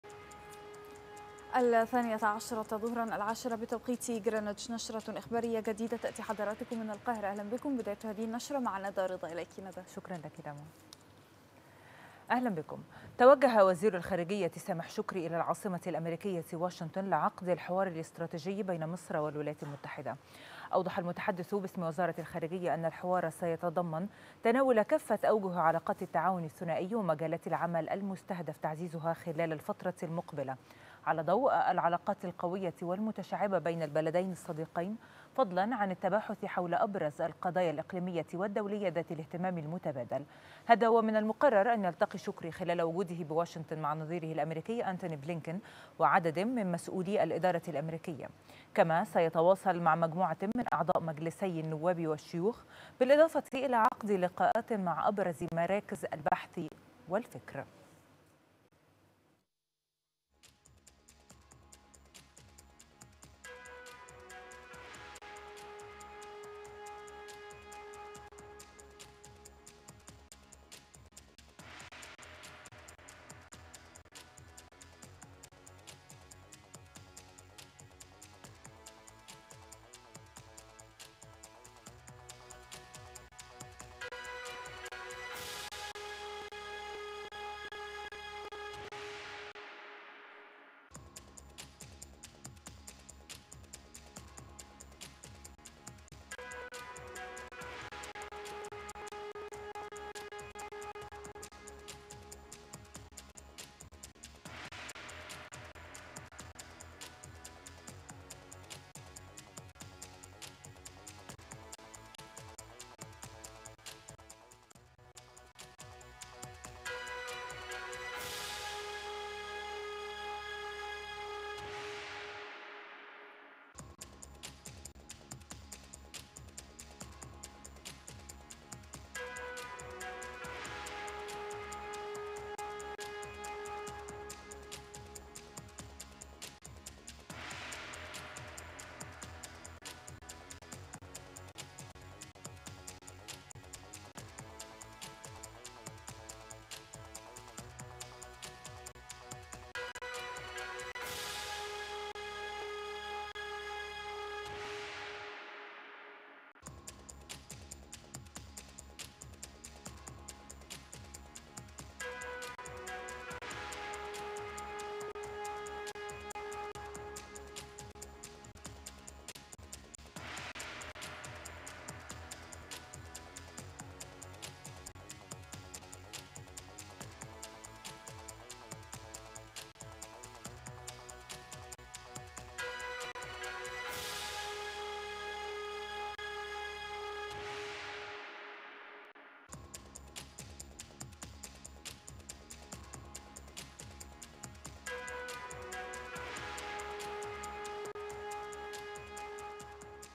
غرفة الأخبار| شكري يتوجه إلى واشنطن لعقد الحوار الاستراتيجي بين مصر وأمريكا
6 نوفمبر 2021